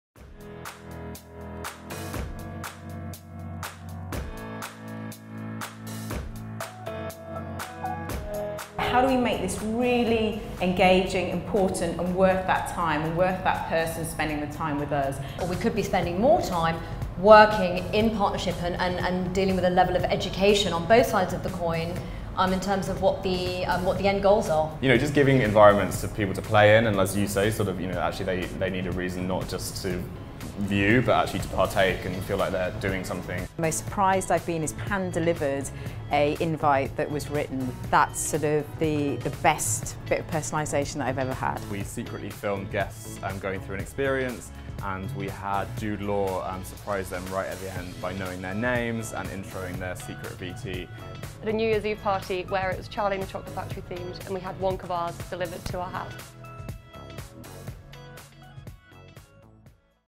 EventLAB Series at LABS Camden House: How Personalise the Live Event
In the latest edition of the EventLAB Series, we got an experienced panel of events and creative professionals together to discuss "Cost-Effective Ways to Personalise the Live Event and Surprise your Audience".

Hosted at the beautiful LABS Camden House, guests experienced an interactive discussion live streamed on social media, inviting audience questions throughout.

The evening also included a delicious array of food and beverage by LABS, melodic and soulful live music from an acoustic lap tap/percussive guitarist courtesy of Musicians inc., a petal parlour for beautiful face and nail decorations from Heaps + Stacks, and of course, plenty of dancing into the night!

Panel participants:
- Moderator: Tom Hall, Editor, Access All Areas
- Charlotte Gentry, Founder & CEO, Pure Events
- Sarah Shilling, CMO, Unlimited Group
- Celine Khor, Head of Brand Experience, John Doe Communications

Mark Forrest, Art Director, The Department

The event was documented by our friends over at London Filmed.
Branding & Signage - Vinyl Impression